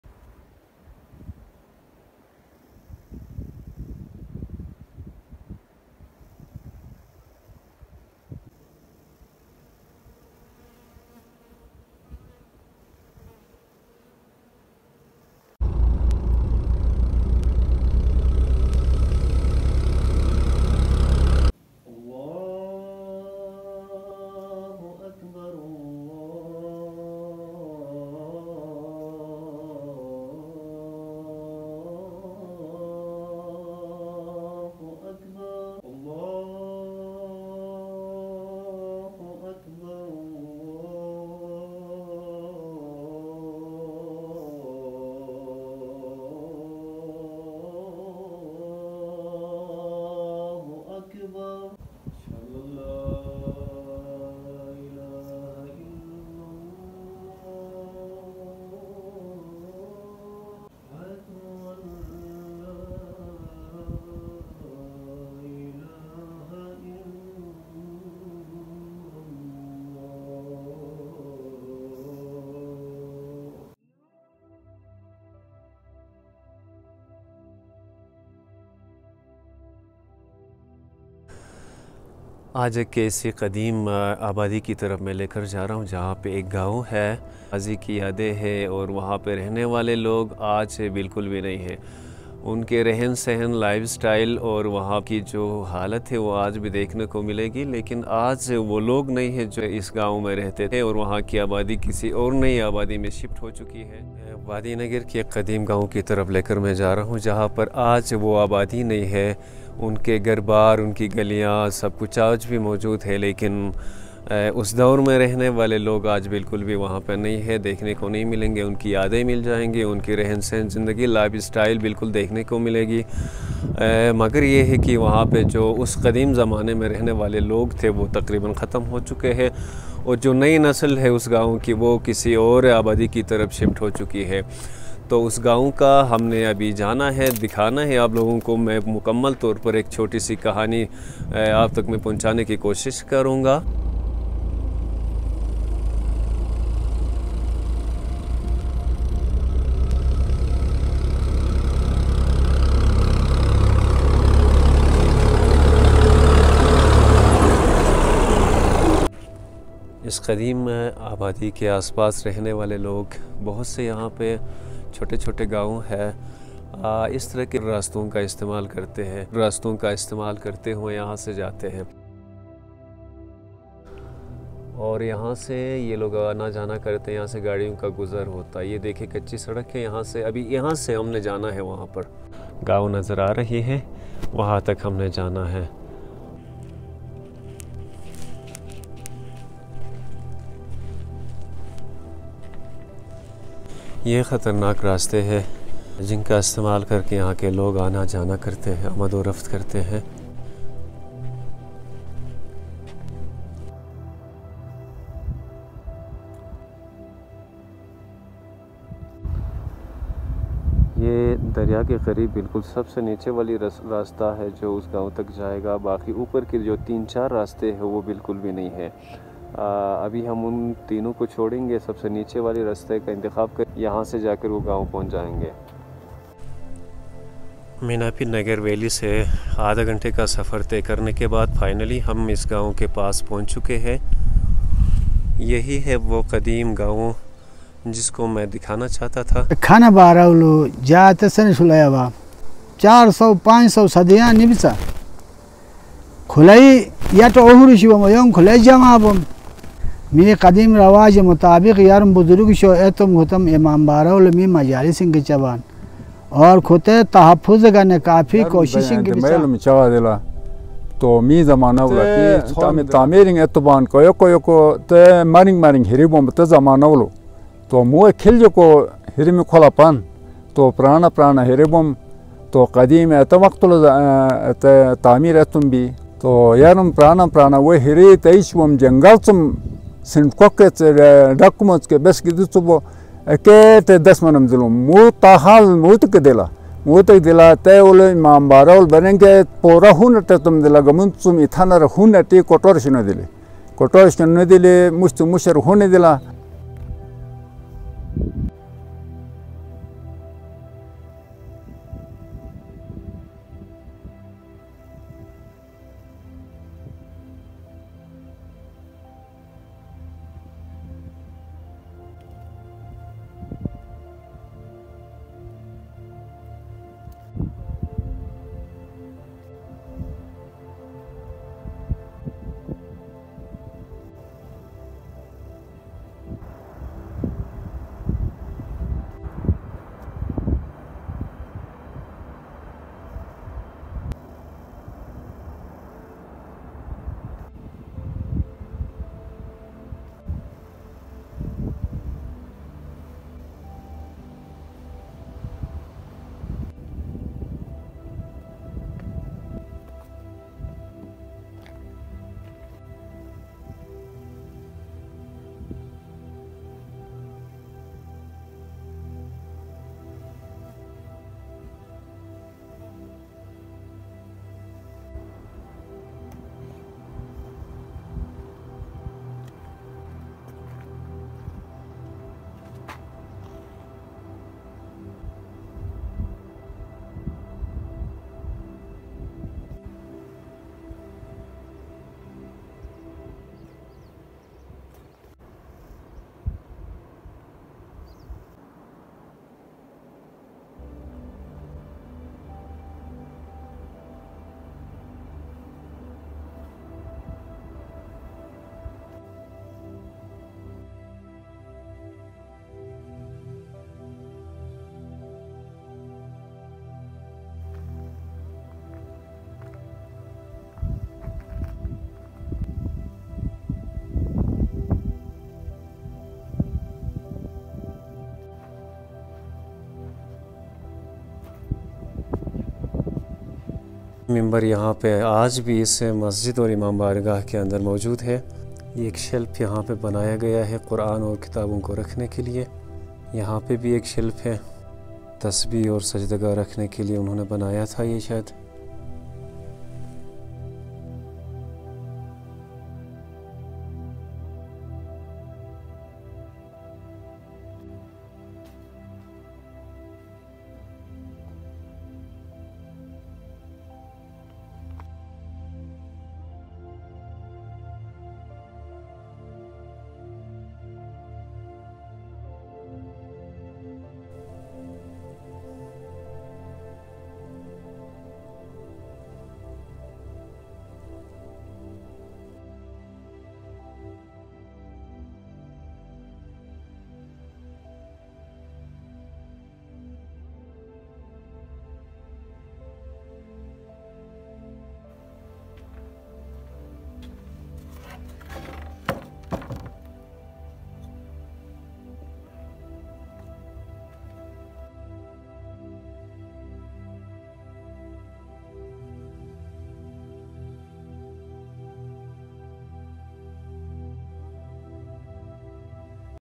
Village Life in pure Mud and Stone Houses in Ancient Village of Nagar Gilgit Baltistan
Welcome to the ancient village of Miachar Nagar valley in Gilgit Baltistan, a place where time seems to have stood still. While the pure mud and stone Mosque, Imam Bargah and houses still stand, the brave people of ancient times are no longer present in this village. The new generation has shifted to another village, leaving behind the remnants of a simpler time. As we explore this abandoned village, we are reminded of a bygone era, and the beauty and simplicity of village life that once was

Please!!